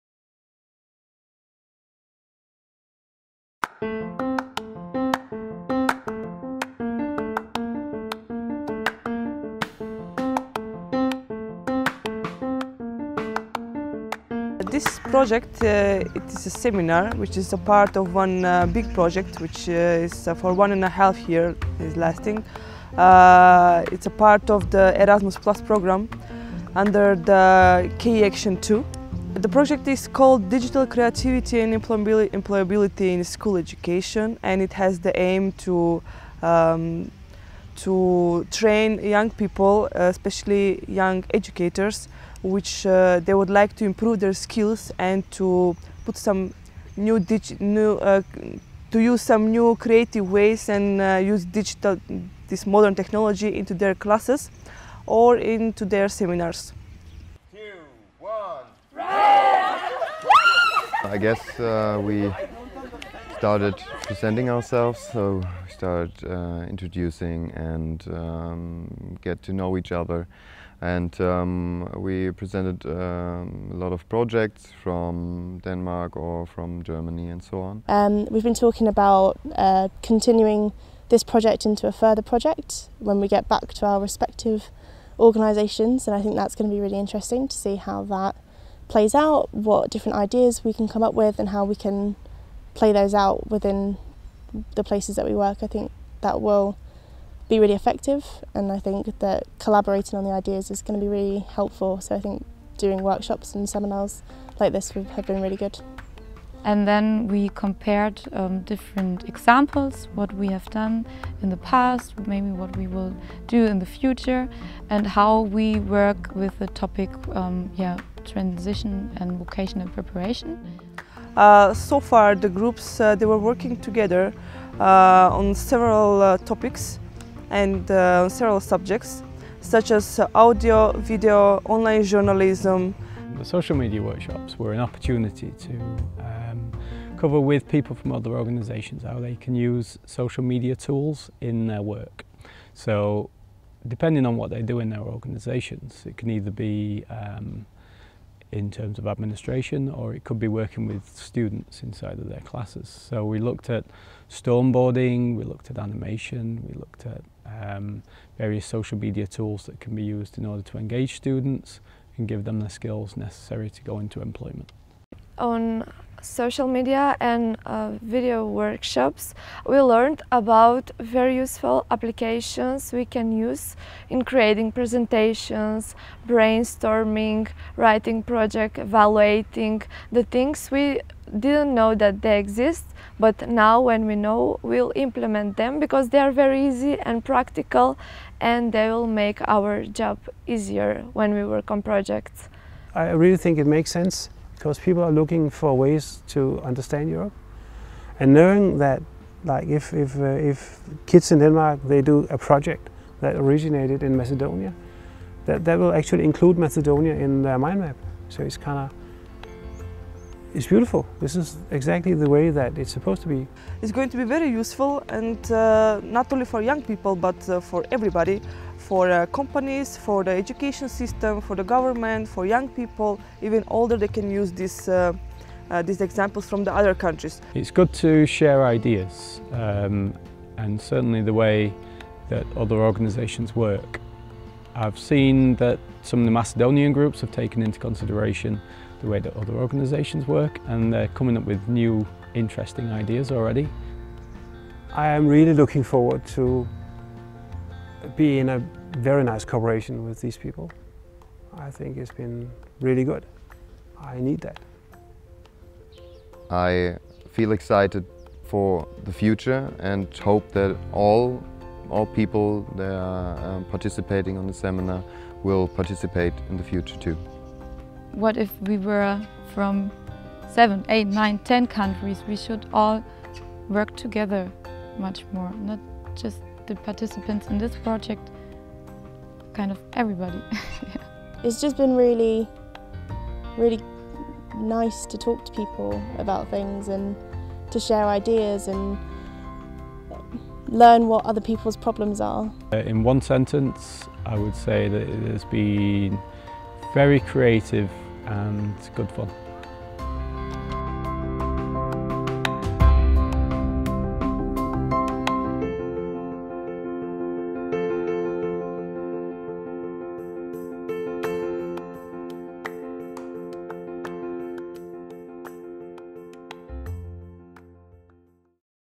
Team Up Youth Macedonia
Employees of Bürgerhaus Bennohaus have visited a training course in Struga and a conference in the Macedonian capital - Skopje as part of the project ‚Team Up Youth‘. Together with media educators from Great Britain, Macedonia, Denmark, the media- and cultural center Bennohaus and the network association ‚Youth4Media‘ develop new curricula and material for the use of media in school education. The meeting took place from 10th till 18th of September 2015.
The project is supported by Erasmus +